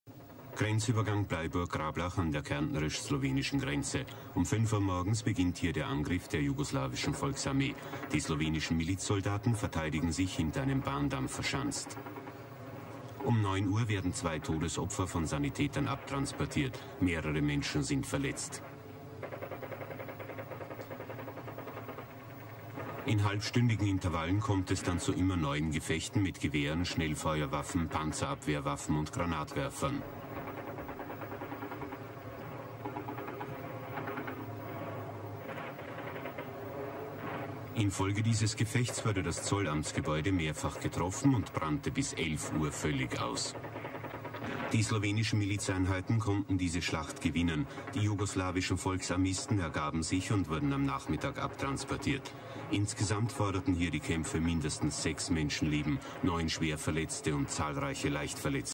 First Yugoslav War Crime - HD Footage [ENG.Subs]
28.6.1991 at Holmec Border Crossing

Footage from Austrian TV News ORF showing how the Yugoslav soldiers came out with a white sheet and raised their hands in front of the Slovenian territorials.

After that, a burst of gunfire was heard, which knocked down part of the soldiers, while others threw themselves on the floor or ran away in a panic.

According to the findings of the investigation, soldiers Simunovic and Jesic died immediately, and Maletic bled after some time.

The footage clearly shows that they were hit from the front and sides, which was confirmed by the pathologist's findings.

Slovenian territorials initially said that JNA soldiers were killed from behind by their superior, who did not allow them to surrender.

Another dissaproved investigation from 1991, led by Drago Kos, showed that the shots came from a different position of reserve police officers, who were not within eyesight with the group of soldiers, but YPA soldiers ran away because they thought they were shooting at them.
They also stated that the members who wanted to surrender all survived the conflict and that three fallen members of the Yugoslav People's Army had died earlier during the conflict.